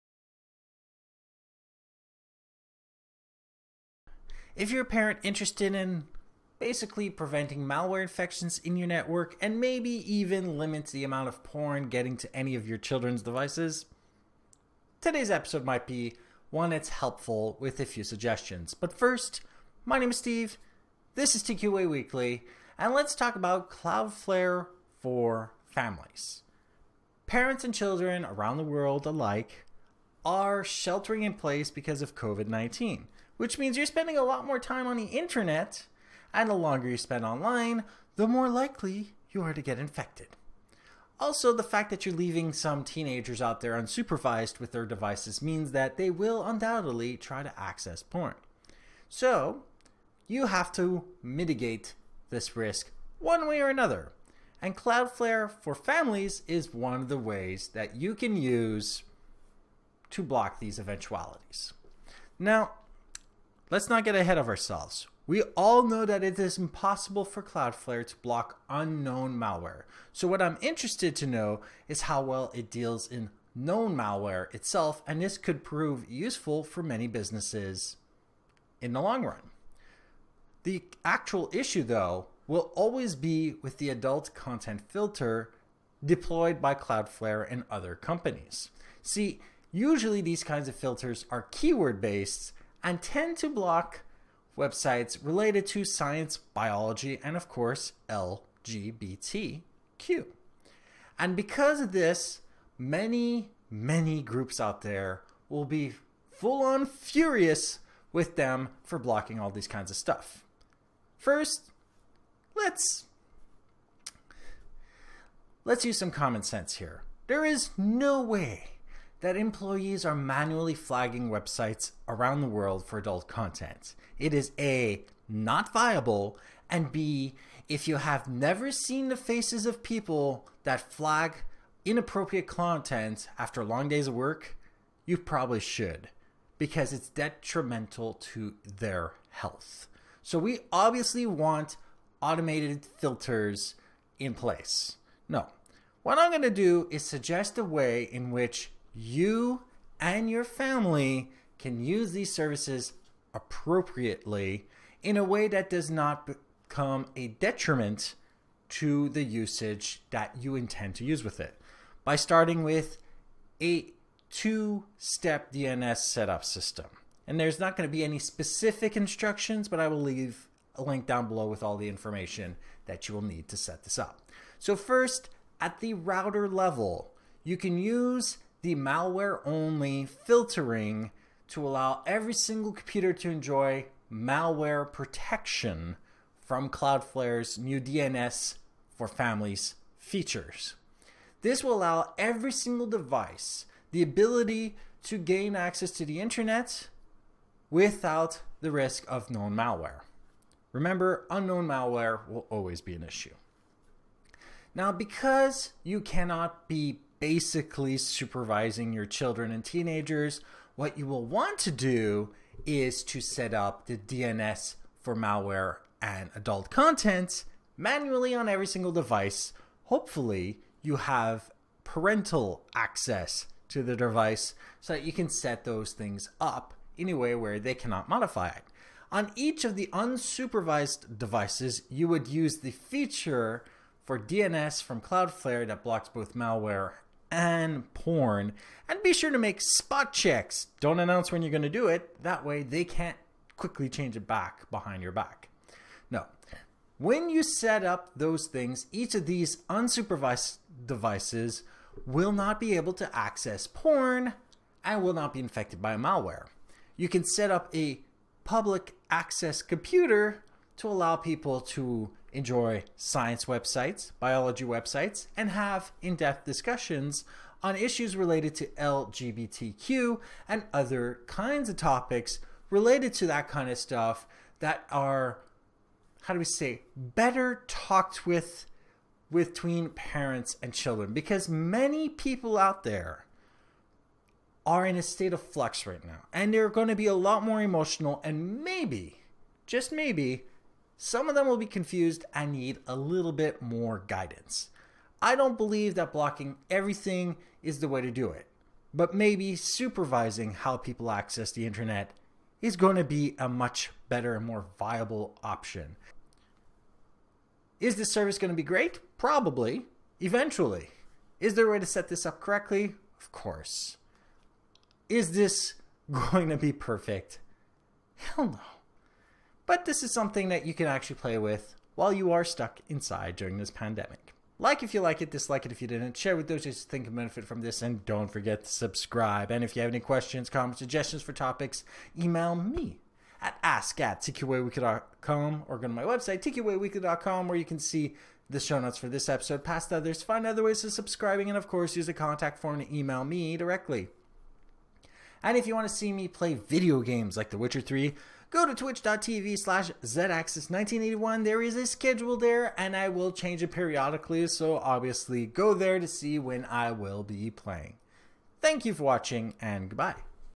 You Can Block Malware and Porn With New DNS Service
Learn about a new service by Cloudflare called Cloudflare for Families

Instructions by Lifehacker:

For IPv4: Add to or replace any IP addresses that may be already listed with:

Malware-blocking

1.1.1.2
1.0.0.2
Malware and adult content-blocking

1.1.1.3
1.0.0.3
For IPv6: Add to or replace any IP addresses that may be already listed with:

Malware-blocking

2606:4700:4700::1112
2606:4700:4700::1002

2606:4700:4700::1113
2606:4700:4700::1003